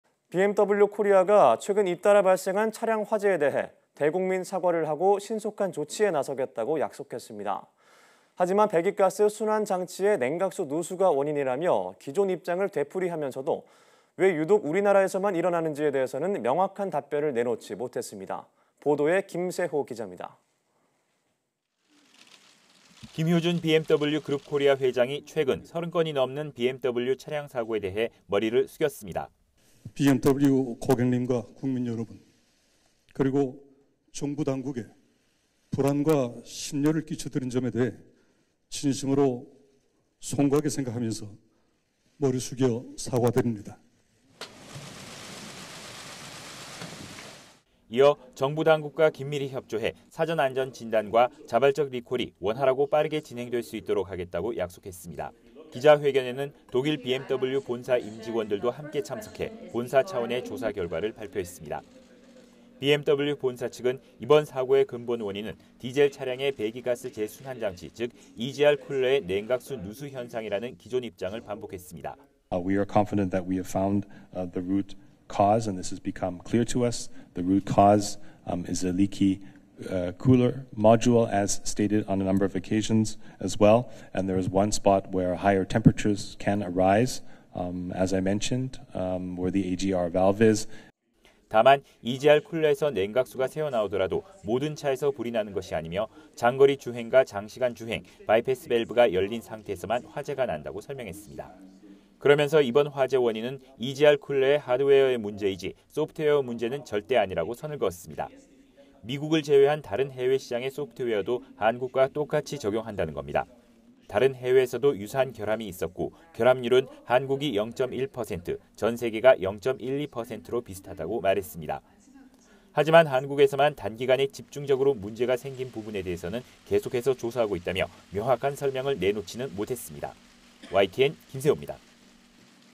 고개숙인 BMW..."EGR 냉각수 누수가 원인" 되풀이 / YTN 사이언스
[앵커]
BMW 코리아가 최근 잇따라 발생한 차량 화재에 대해 대국민 사과를 하며 신속한 조치에 나서겠다고 약속했습니다.

하지만 배기가스 순환 장치의 냉각수 누수가 원인이라며 기존 입장을 되풀이하면서도, 왜 유독 우리나라에서만 일어나는지에 대해서는 명확한 답변을 내놓지 못했습니다.

보도에 김세호 기자입니다.

[기자]
김효준 BMW 그룹 코리아 회장이 최근 30건이 넘는 BMW 차량 사고에 대해 머리를 숙였습니다.

[김효준 / BMW 그룹 코리아 회장 : BMW 고객님과 국민 여러분 그리고 정부 당국에 불안과 심려를 끼쳐 드린 점에 대해 진심으로 송구하게 생각하면서 머리 숙여 사과드립니다.]

이어 정부 당국과 긴밀히 협조해 사전 안전 진단과 자발적 리콜이 원활하고 빠르게 진행될 수 있도록 하겠다고 약속했습니다.

기자회견에는 독일 BMW 본사 임원진들도 함께 참석해 본사 차원의 조사 결과를 발표했습니다.

BMW 본사 측은 이번 사고의 근본 원인은 디젤 차량의 배기가스 재순환 장치, 즉 EGR 쿨러의 냉각수 누수 현상이라는 기존입장을 반복했습니다.

[글렌 슈미트 / BMW그룹 기업홍보 총괄 : EGR 쿨러쪽의 누수가 근본 원인이라고 결론을 내릴 수 있었습니다. 그리고 엔진실에서 고온으로 올라가면서 화재의 위험까지 줄 수 있는 부분은 EGR 바이패스 부분이라는 것까지 파악했습니다.]

다만 EGR 쿨러에서 냉각수가 새어 나오더라도 모든 차에서 불이 나는 것이 아니며, 장거리 주행과 장시간 주행, 바이패스 밸브가 열린 상태에서만 화재가 난다고 설명했습니다.

그러면서 이번 화재 원인은 EGR 쿨러의 하드웨어 문제이지, 소프트웨어 문제는 절대 아니라고 선을 그었습니다.

미국을 제외한 다른 해외시장의 소프트웨어도 한국과 똑같이 적용한다는 겁니다.

다른 해외에서도 유사한 결함이 있었고, 결함률은 한국이 0.1%, 전 세계가 0.12%로 비슷하다고 말했습니다.

하지만 한국에서만 단기간에 집중적으로 문제가 생긴 부분에 대해서는 계속해서 조사하고 있다며 명확한 설명을 내놓지는 못했습니다.

YTN 김세호입니다.